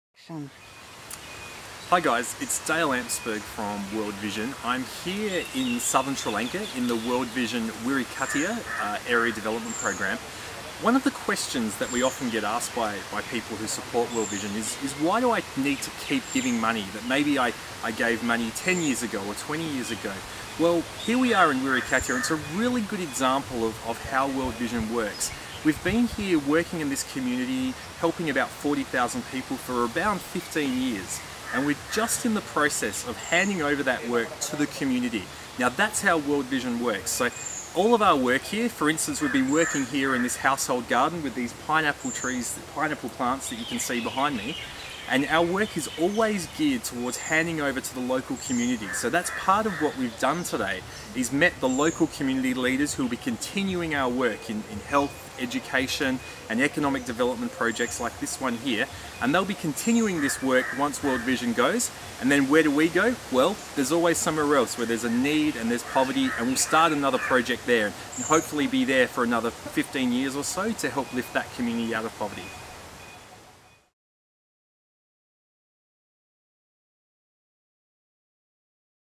Why do I need to keep giving money?
World Vision supporters often ask, "Why do I need to keep giving money?"

Whilst in Sri Lanka, World Vision staff member Dale answers by sharing a little about our Child Sponsorship model for development empowering children and communities.

Discover more about your Child Sponsorship, log into My World Vision

Not a child sponsor? Discover the future with World Vision today